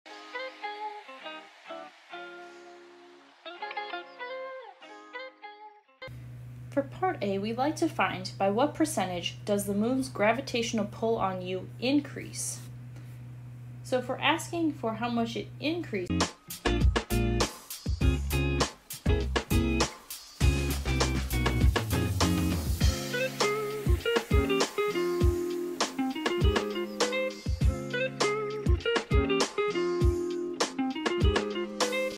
Moon effect: Some people believe that the Moon controls their activities. If the Moon moves from be…
Moon effect: Some people believe that the Moon controls their activities. If the Moon moves from being directly o the opposite side of Earth from vou to being directly overhead_ what percentage does (a) the Moon x27; gravitational Pull , you increase and (b) your weight (as measured on scale_ decrease? Assume tnat the Earth-Moon (center-to-center) distance is 3.82 10* m; Earth x27; radius 6.37 106 Moon x27; mass is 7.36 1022 and Earth quot; mass is 5.98 x 10?- (a) Number Units (b) Number Units